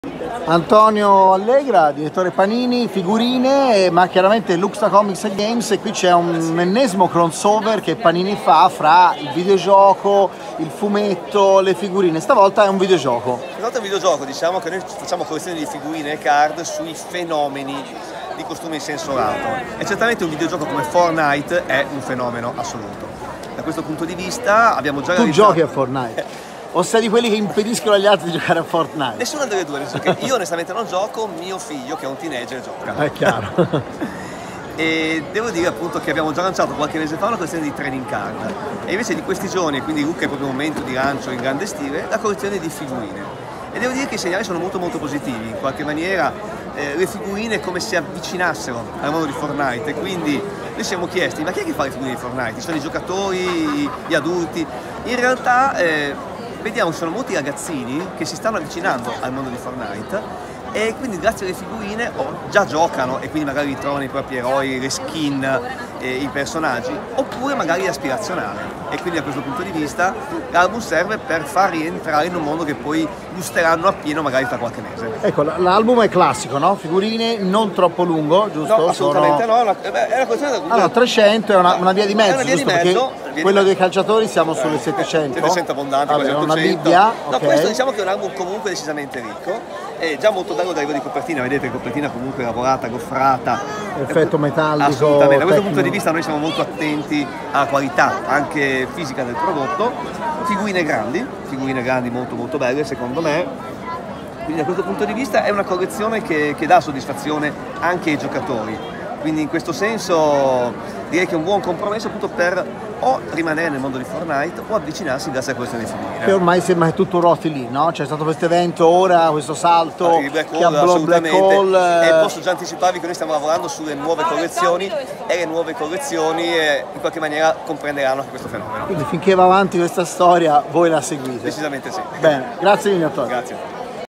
Figurine Fortnite , carta e virtuale si scambiano di posto
Nuova edizione di figurine dedicate a Fortnite da parte di Panini ed è l’occasione per una riflessione su chi fa queste raccolte e perché carta (di qualità) e sfide virtuali non sono mondi così distanti